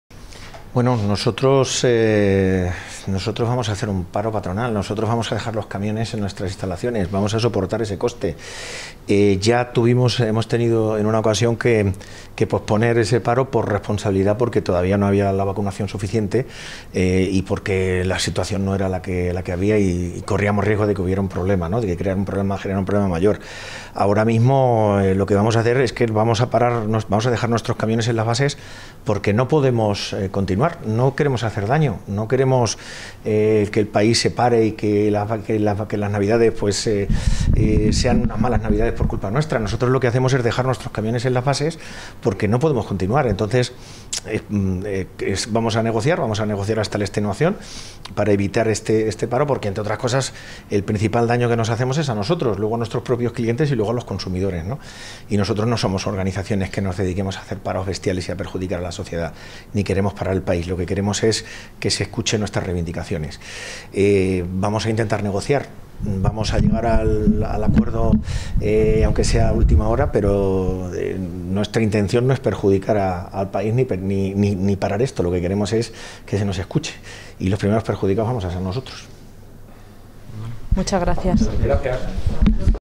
Patronal del Transporte de C-LM confía en el diálogo para evitar la huelga
El presidente de la Federación de Transporte de Mercancías de Castilla-La Mancha (FETCAM), Carlos Marín, se ha mostrado confiado a unas semanas del parón anunciado por el sector en que el diálogo con el Ministerio llegue a buen puerto y se pueda evitar la huelga.